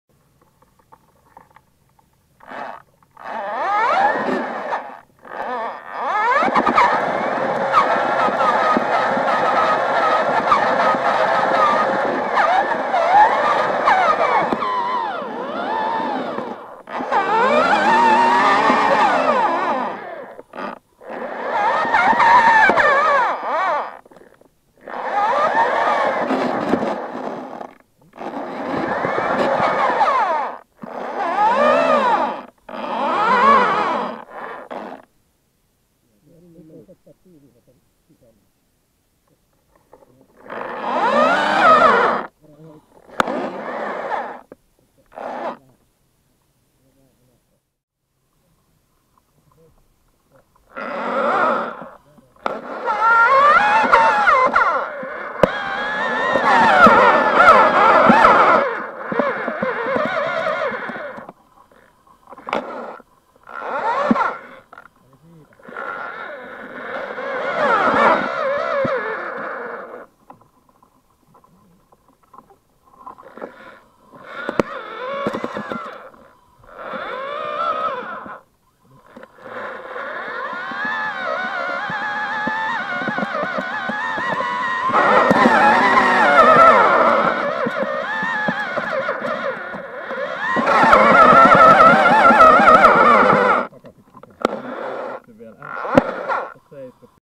Badger Tunnel Robot - the first drive into a tunnel and back
Badger Tunnel Robot is a student project aiming to film badgers dens. Students at Aalto University have built a robot which is able to drive to the den and back. This video is the first test drive of the robot.